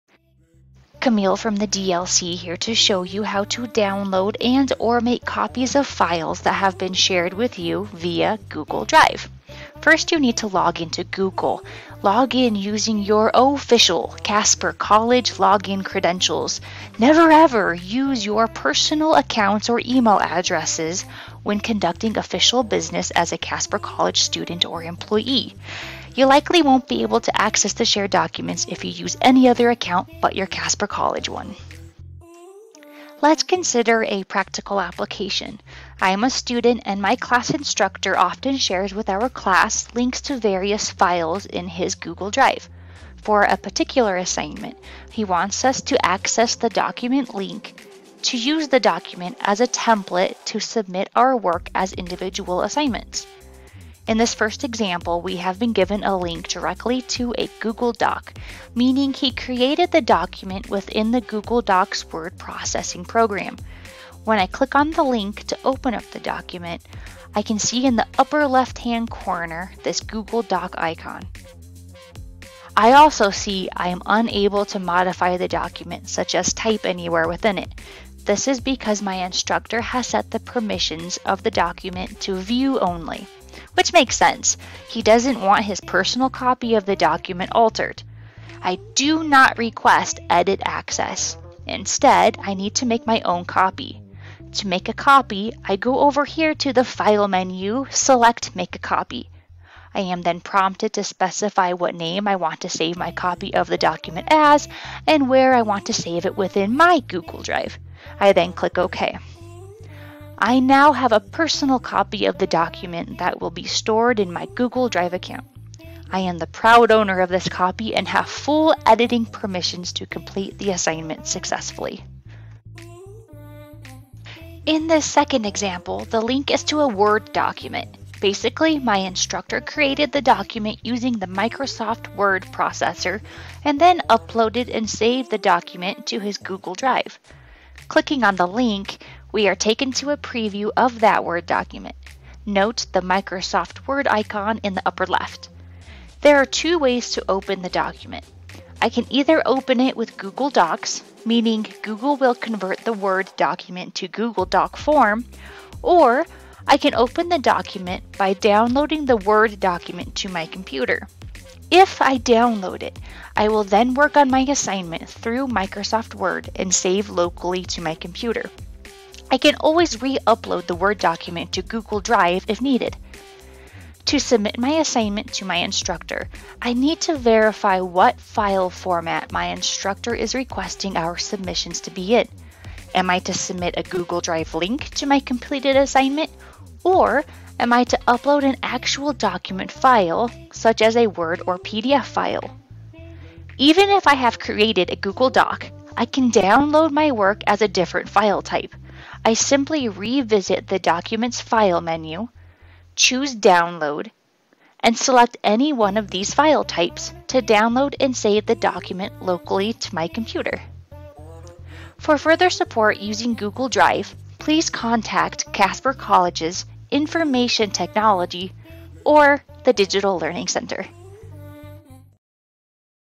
GoogleDrive Shared Documents Access and Make a Personal Copy student perspective
1) Accessing a View Only GoogleDrive file that has been shared with you via a link, such as in your Moodle course.
2) Making your own personal copy of the file in order to make edits (such as when your instructor wants you to use the document as a template for an assignment.)
3) Downloading your GoogleDoc to another file format (Word, PDF, etc.).